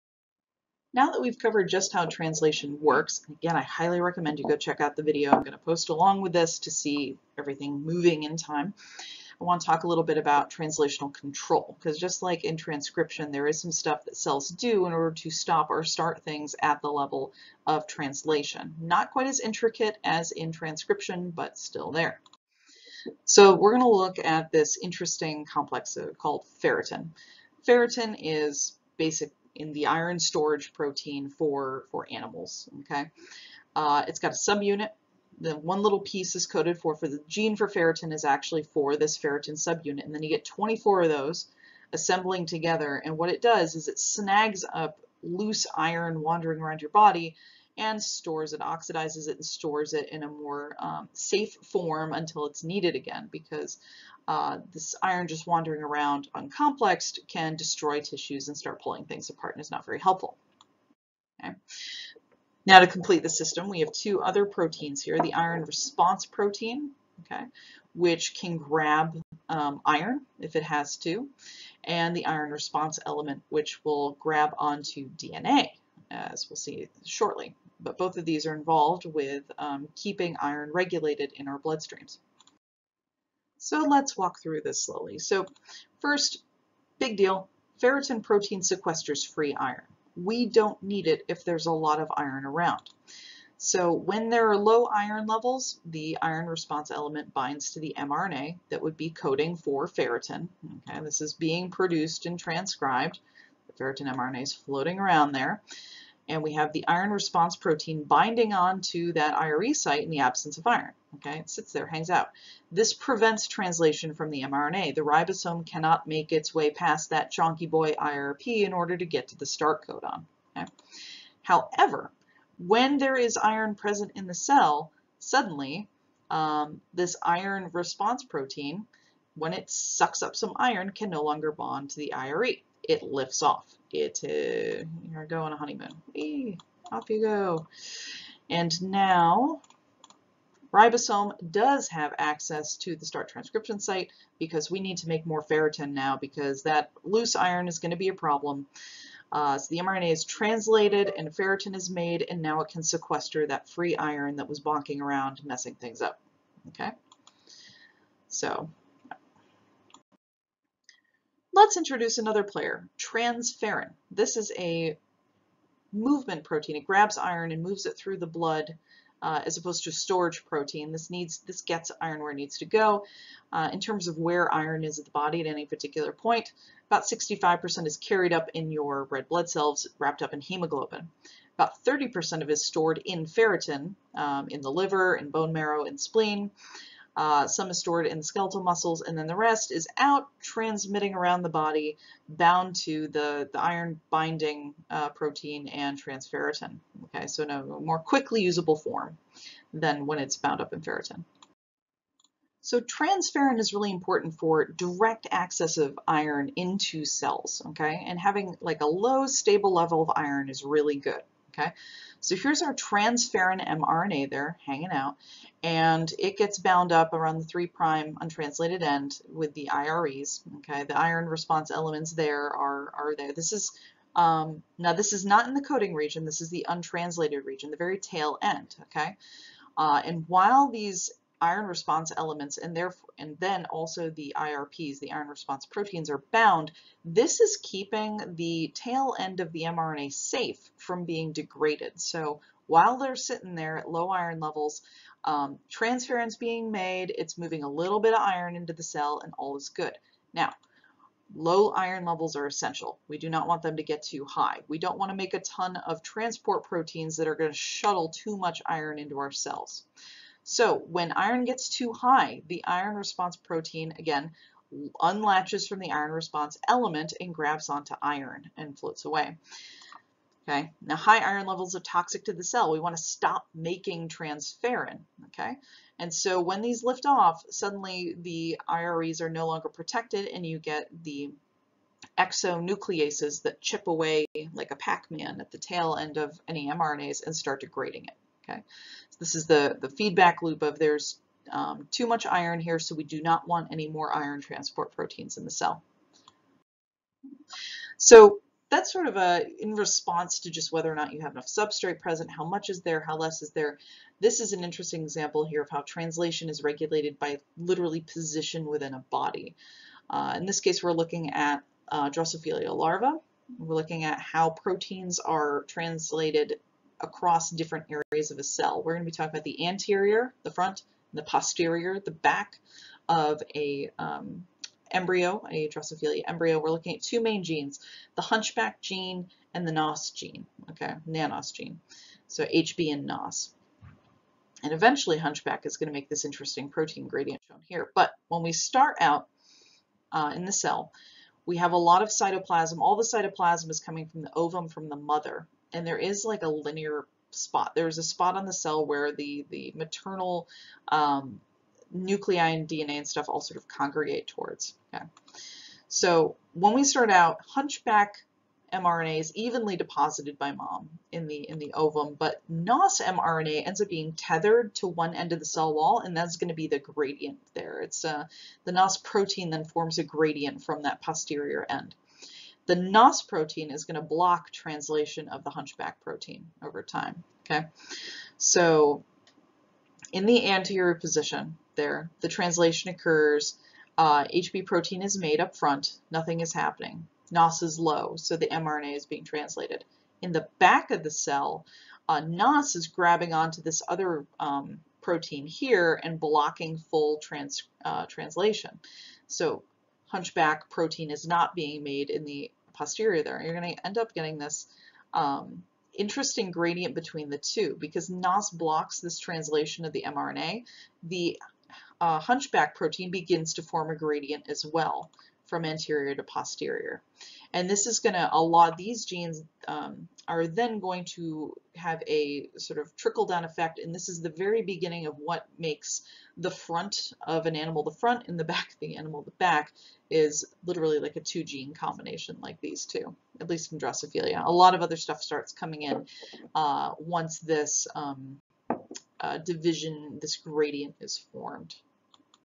Ch.13 - Translation - Part 2 - Translational Control
More translation! Ferretin and transferrin.